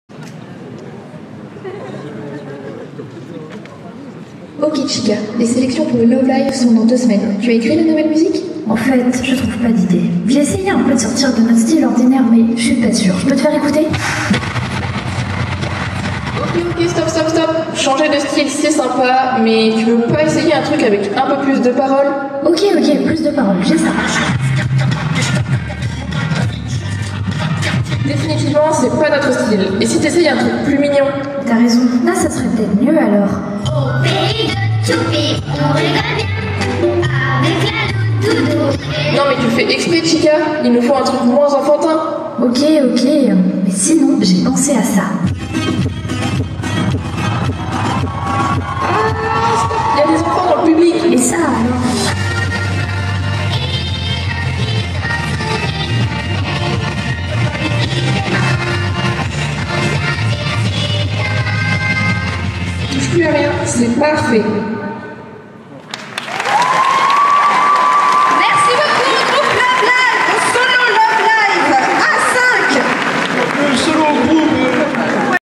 Japan Expo 2022 - Concours Cosplay Vendredi - 05 - Love Live - Chika
Cosplay Contest organized during the Friday of Japan Expo in July 2022
at The Park des Exposition de Villepinte
Concours Cosplay organisé pendant le vendredi de Japan Expo en juillet 2022
au parc des Exposition de Villepinte

Remerciements divers:
* à l'équipe du festival.
* à l'association gérant les concours.
* aux cosplayeurs.

-- Various links